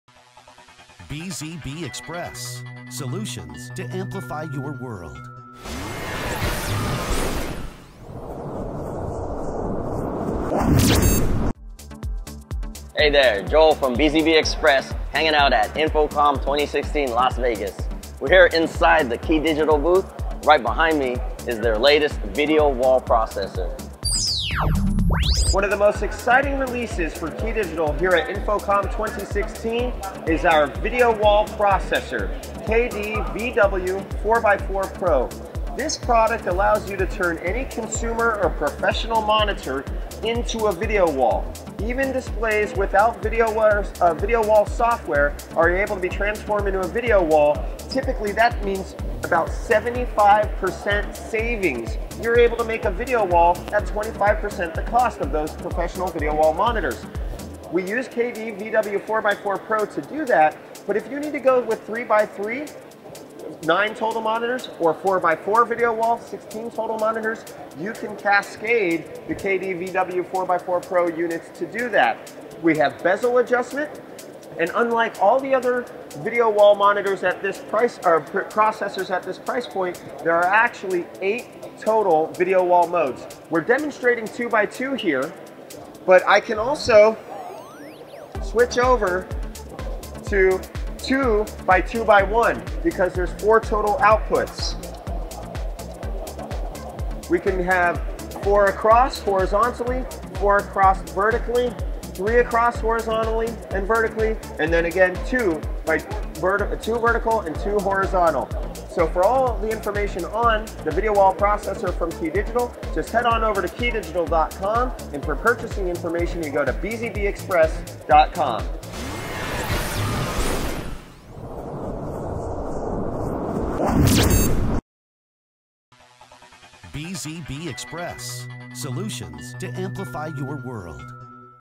How to Transform Any HDTV Into a Professional Video Wall
The versatile KD-VW4x4Pro can turn any consumer or professional display into a video wall while offering eight modes. In addition, HDMI matrix switching is seamless with this unit and it has 4 HDMI/analog inputs to 4 HDMI outputs. Lifetime Warranty available.

Thanks to Jonathon Ferry and the Key Digital staff for allowing our crew to shoot the video at their awesome booth from InfoComm 2016.